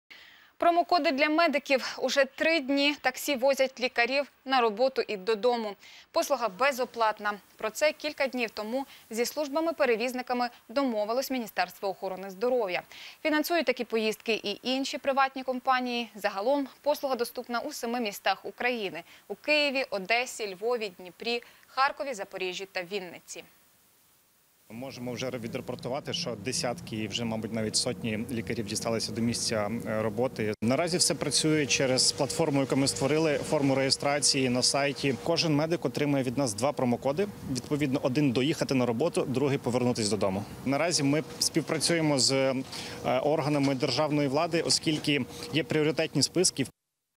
Медики на таксі
Перевізники задарма доправлятимуть медиків на роботу
СТН (25.03.20)